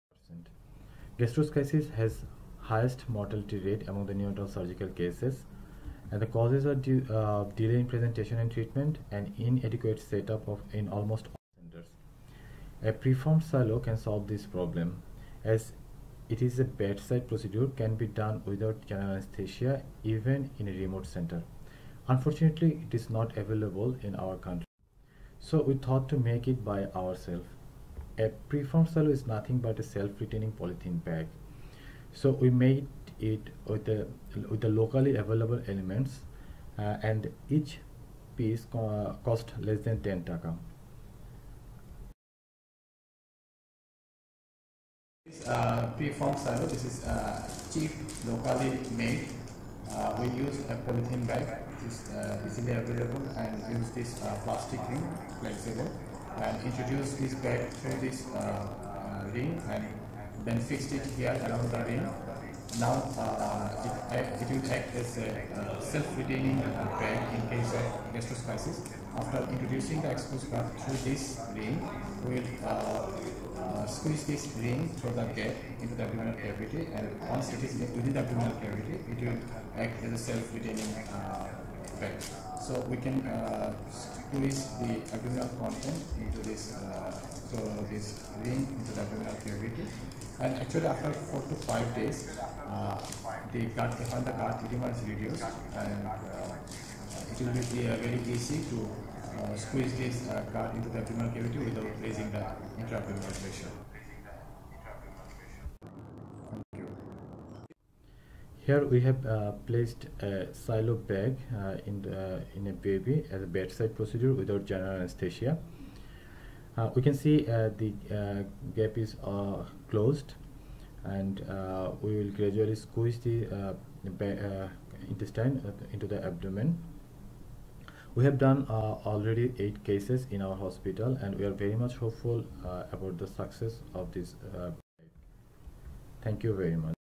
Locally made cost effective preformed Silo can improve the survival of Gastroschisis by 90%!!!
Demonstrate locally made, cost-effective preformed Silo for the treatment of Gastroschisis!